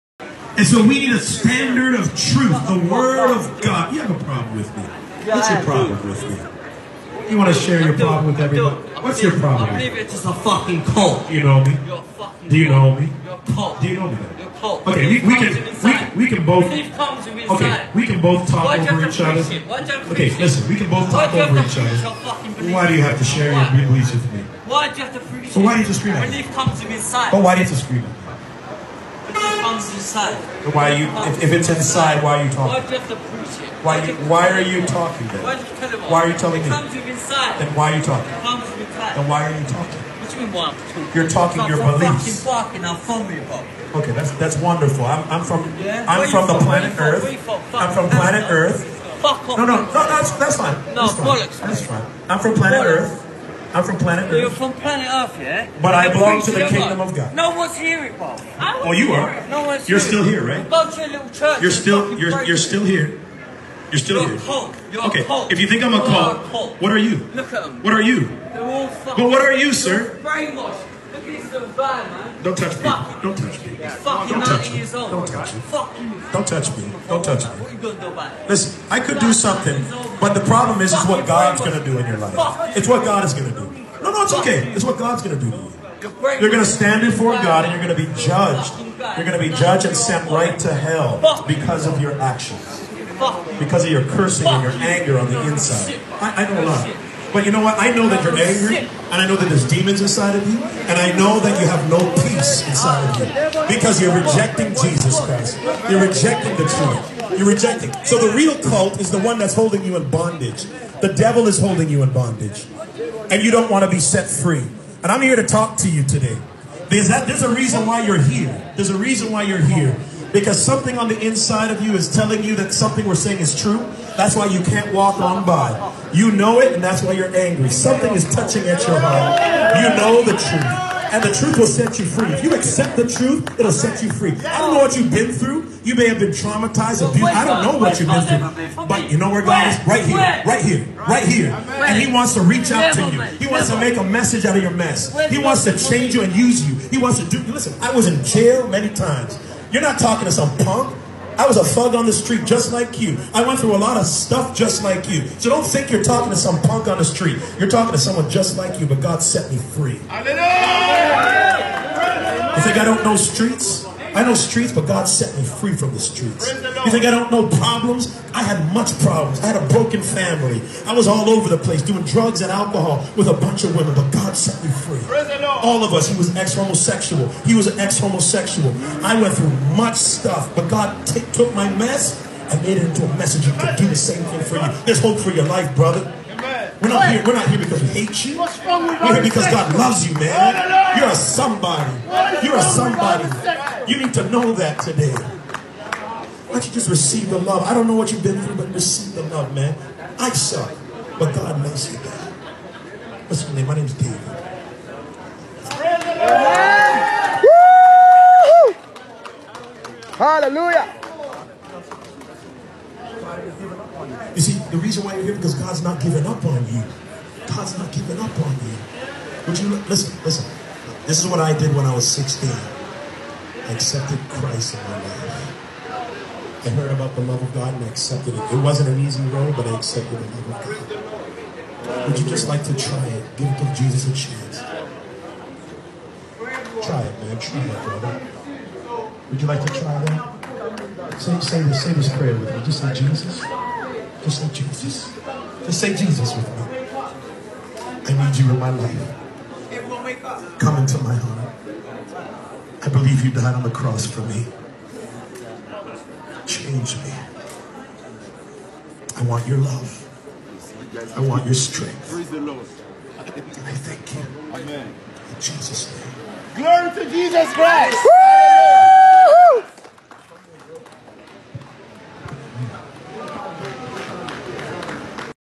Attacked for preaching, but Preaching the Word of God is A lifestyle backed up by God
Even the gangster surrendered to Jesus Christ